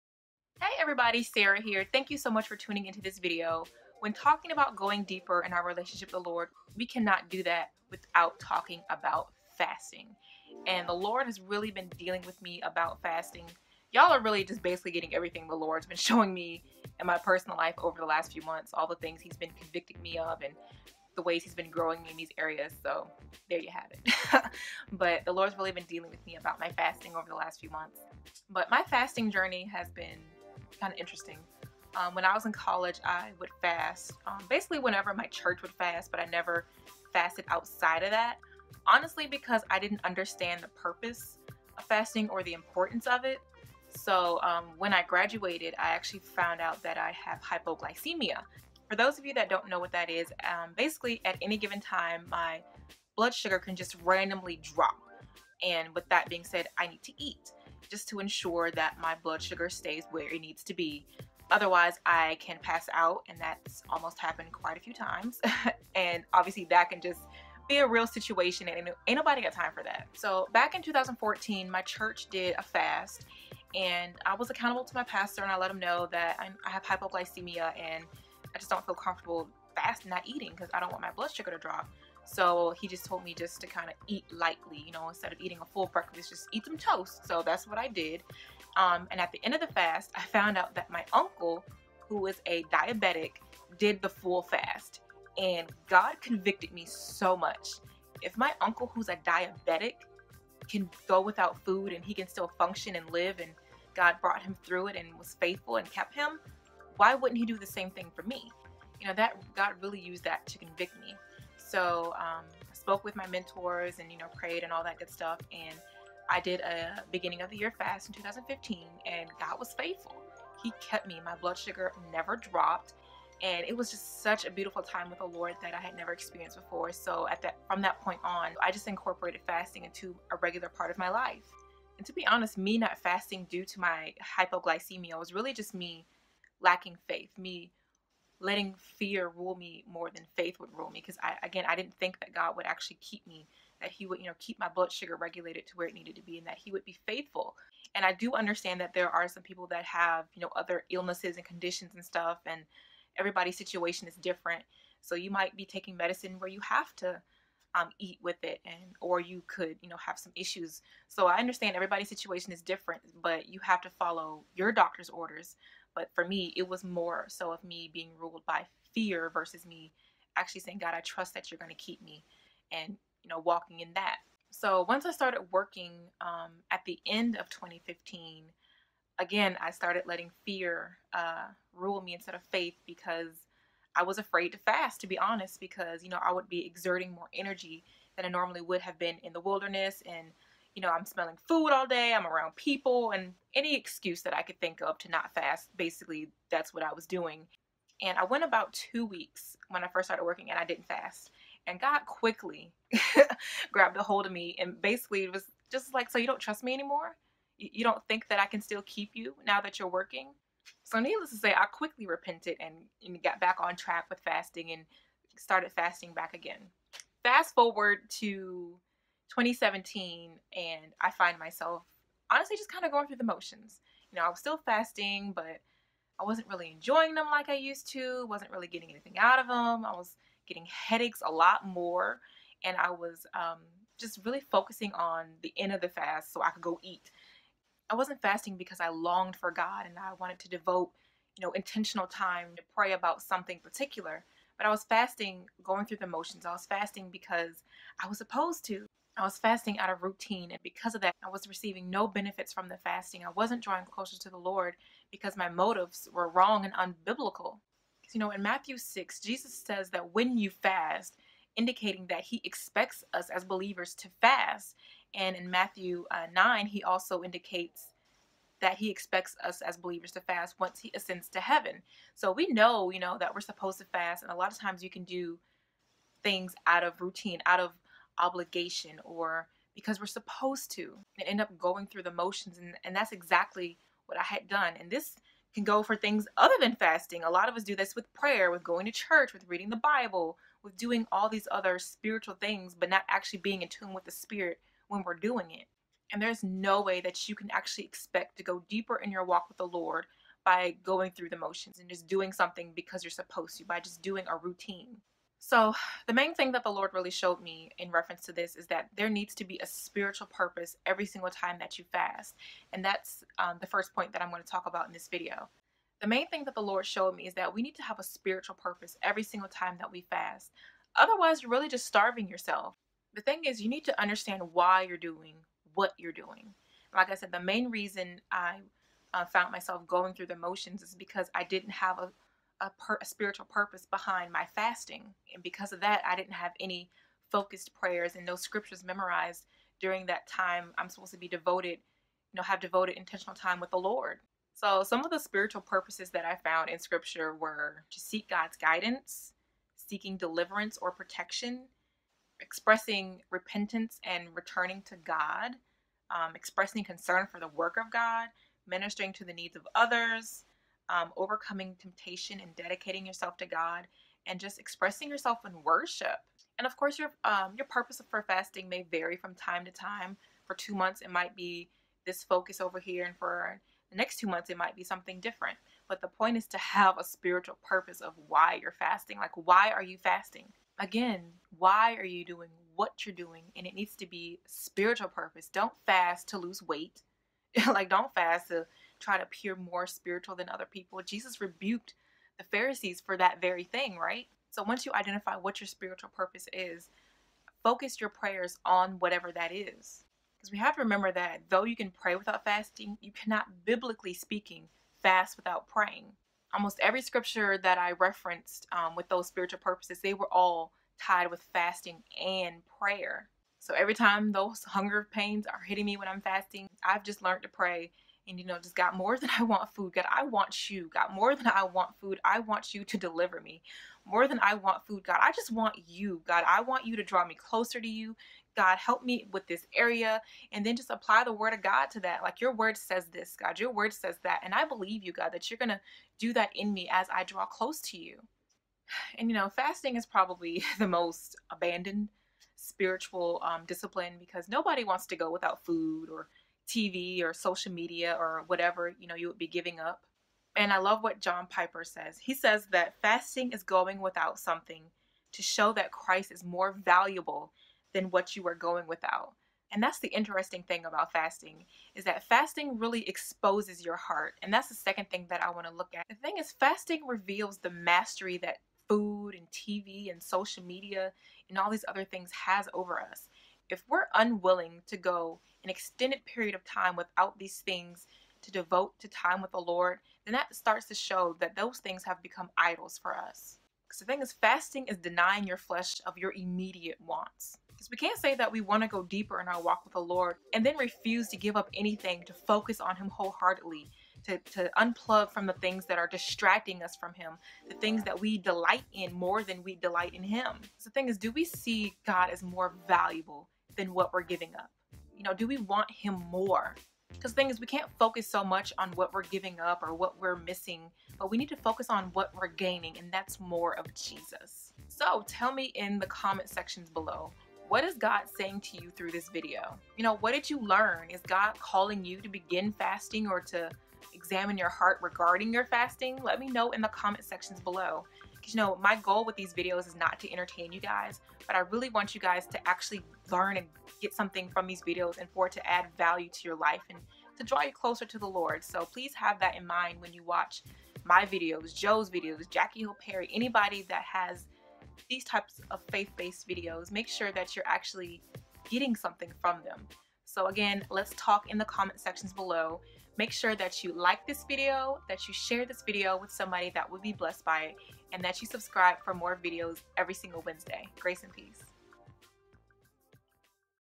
Stop Going Through the Motions | Going Deeper with Fasting
In this video, I share my journey of fasting over the years & why it’s important to understand why we do whatever it is that we’re doing. We can’t expect to go deeper in our walk with God if we’re just going through the motions.

IG & Twitter: @swpministry